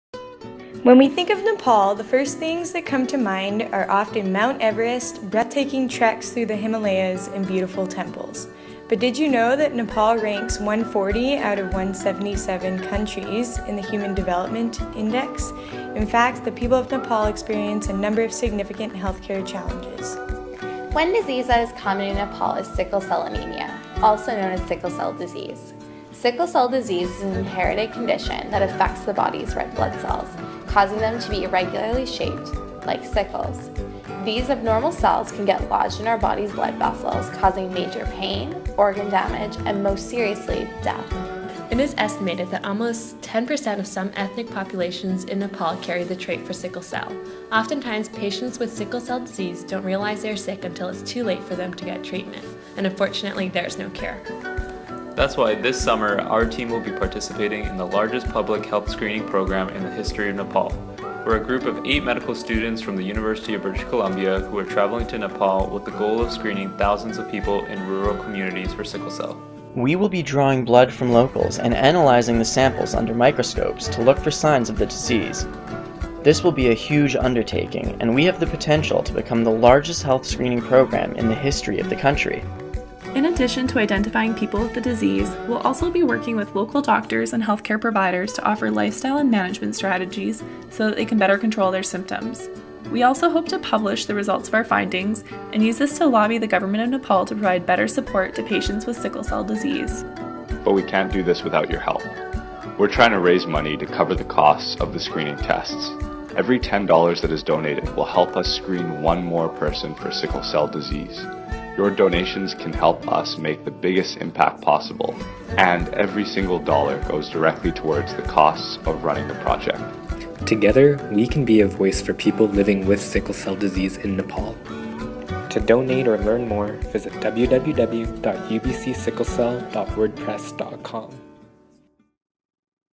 What is the UBC Sickle Cell Screening Project?
Eight medical students from the University of British Columbia in Vancouver, Canada are travelling to rural Nepal this summer to participate in the largest health screen in the Nepali history.